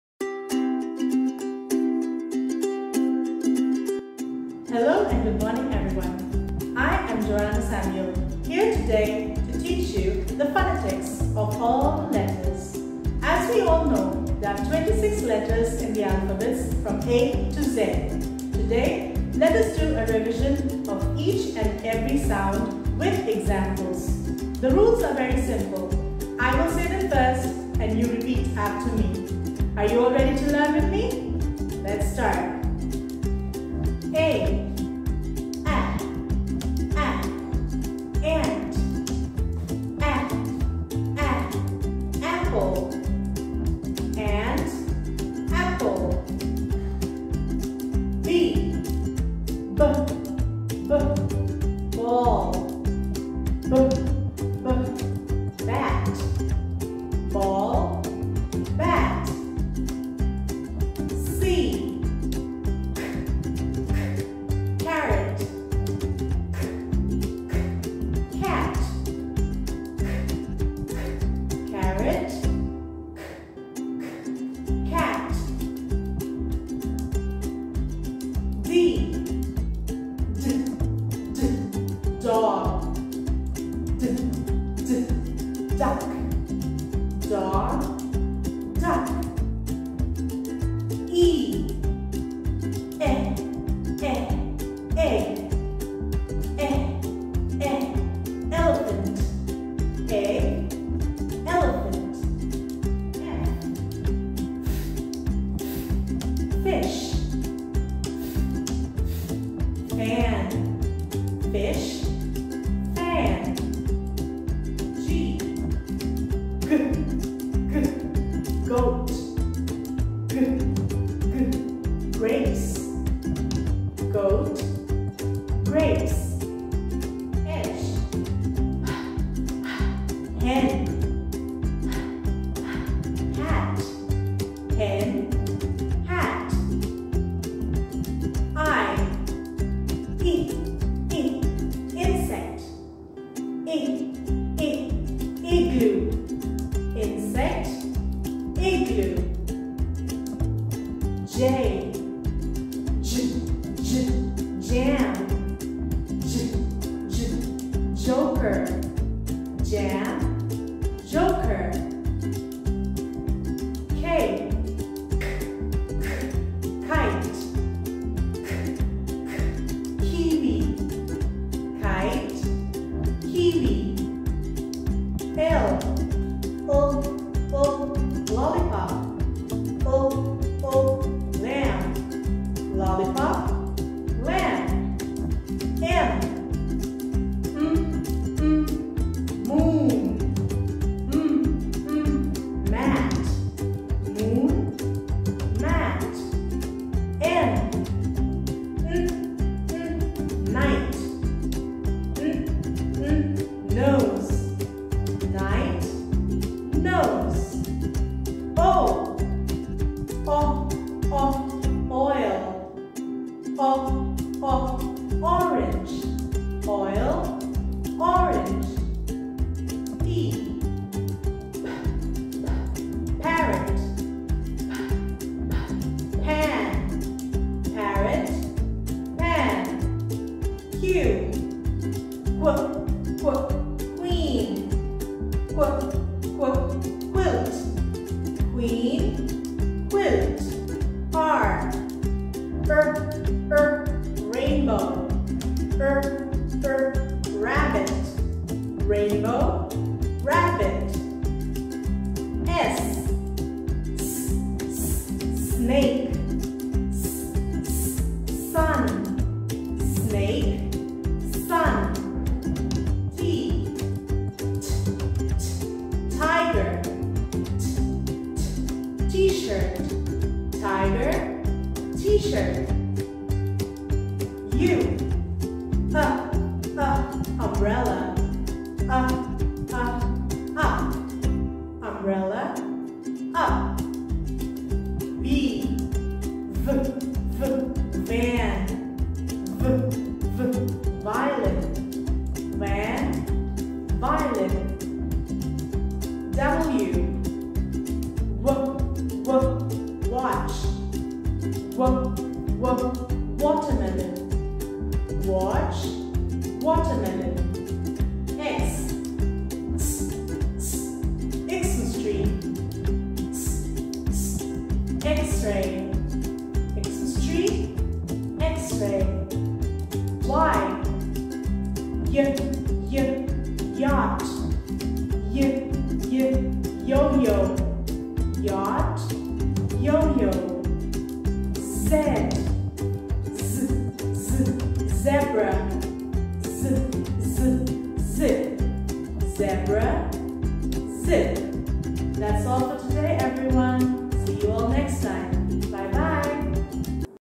Phonics
The video Focuses on letting the kindergarten children understand and learn the various sounds of the english alphabets